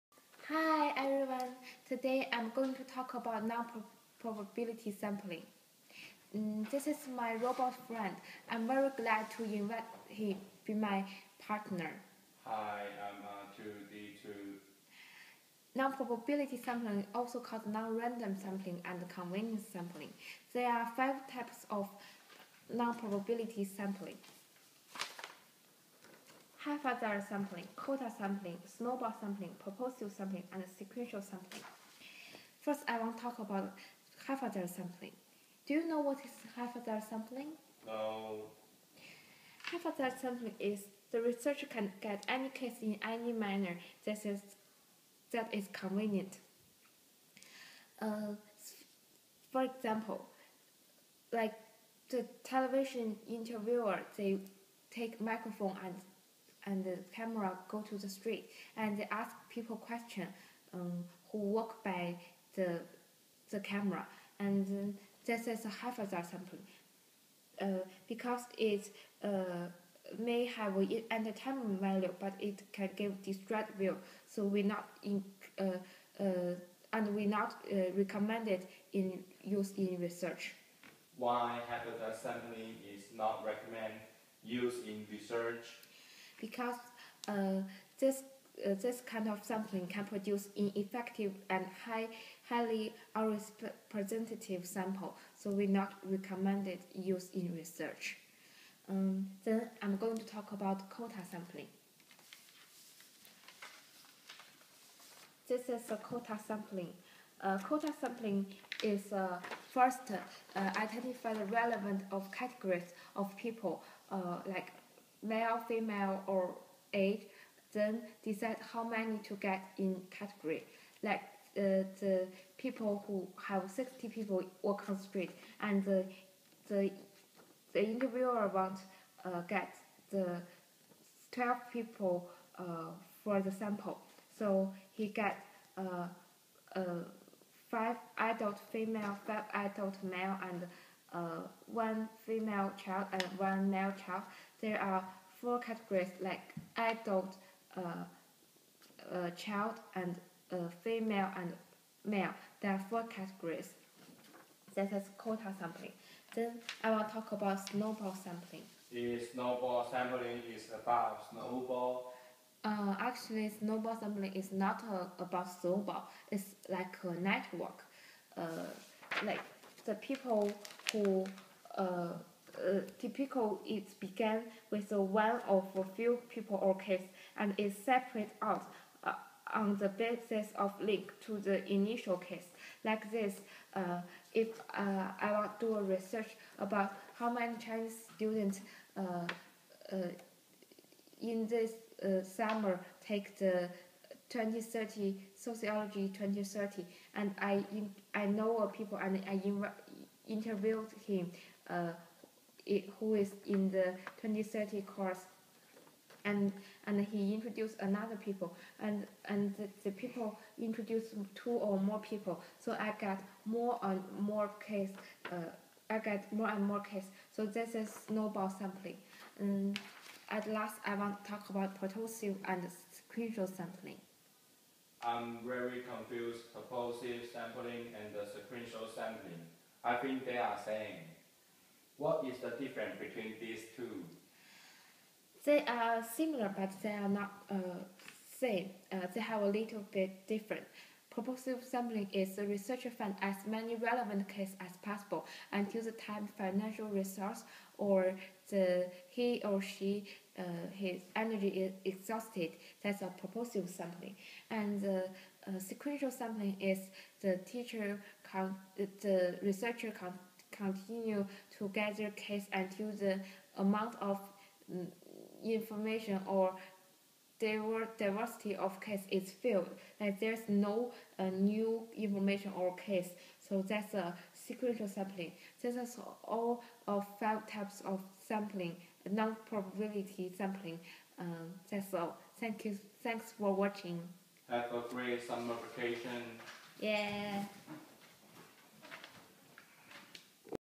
Five Types of Nonprobability Sampling
Haphazard, Quota, Snowball, Purposive, Sequential.